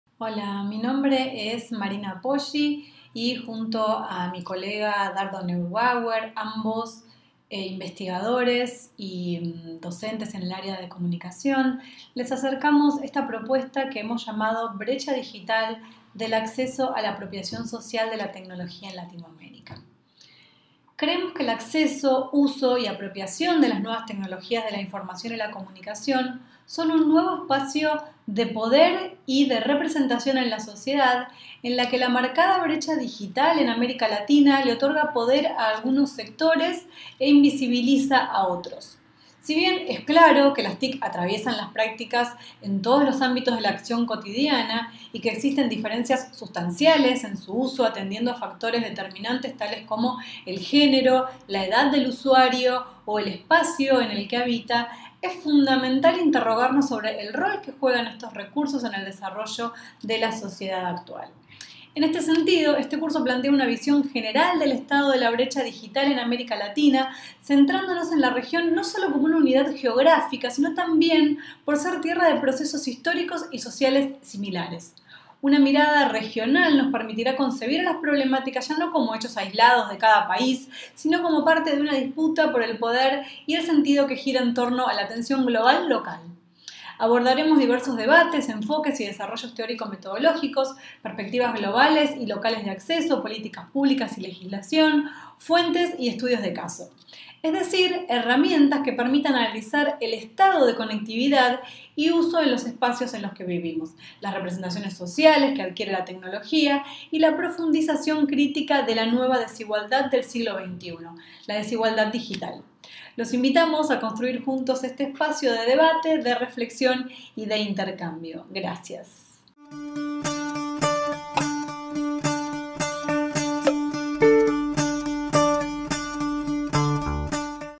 Brecha digital: del acceso a la apropiación social de la tecnología en Latinoamérica
Presentación del curso:

El acceso, uso y apropiación de las TIC y la comunicación se erigen como un nuevo espacio de poder y de representación en la sociedad, en el que la marcada brecha digital en América Latina le otorga poder a algunos sectores e invisibilidad a otros.
Si bien es claro que las TIC atraviesan las prácticas en todos los ámbitos de acción cotidianos, y que existen diferencias sustanciales en el uso atendiendo a factores determinantes tales como el género, la edad del usuario o el espacio que habita, es fundamental interrogarnos sobre del rol que juegan estos recursos en el desarrollo de la sociedad actual.
En este sentido, este curso plantea una visión general del estado de la brecha digital en América Latina, centrándonos en la región no sólo como unidad geográfica sino también por ser tierra de procesos históricos y sociales similares. Una mirada regional nos permitirá concebir a las problemáticas ya no como hechos aislados de cada país sino como parte de una disputa por el poder y el sentido que gira en torno a la tensión global-local.
Abordaremos diversos debates, enfoques y desarrollos teórico-metodológicos, perspectivas globales y locales de acceso, políticas públicas y legislación, fuentes y estudios de caso. Es decir, herramientas que permitan analizar el estado de conectividad y uso en los espacios en los que vivimos, las representaciones sociales que adquiere la tecnología y la profundización crítica de la nueva desigualdad del siglo XXI: la desigualdad digital.
Contenidos:

Debates, enfoques y desarrollos teórico-metodológicos.
Perspectivas de TIC y virtualidad. Educación virtual.
Perspectivas globales y locales de acceso.
Perspectivas de género.
Políticas públicas y legislación. Fuentes y metodologías.
Sabiduría digital.
Nativos e inmigrantes digitales. Estudios de caso.